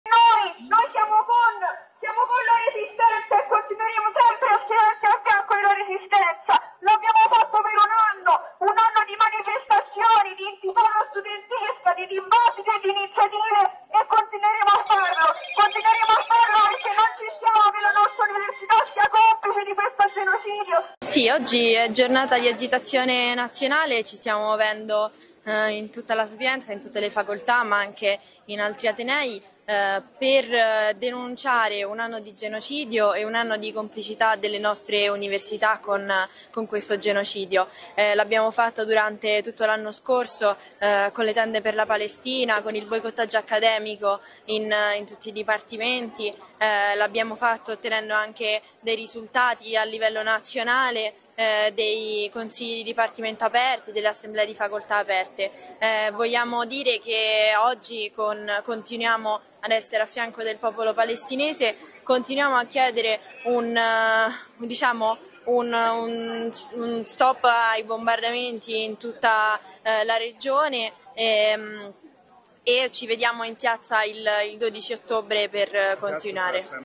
Giovani pro Palestina, nuovo flash mob alla Sapienza
Nuovi azioni di protesta all'università Sapienza di Roma in occasione della "Giornata di agitazione studentesca nazionale per la Palestina". Flash mob sono stati organizzati nei principali atenei di tutta Italia. La breve manifestazione con striscione e bandiera palestinese è stata anche l'occasione per annunciare la prossima mobilitazione pro Pal prevista per sabato 12 ottobre.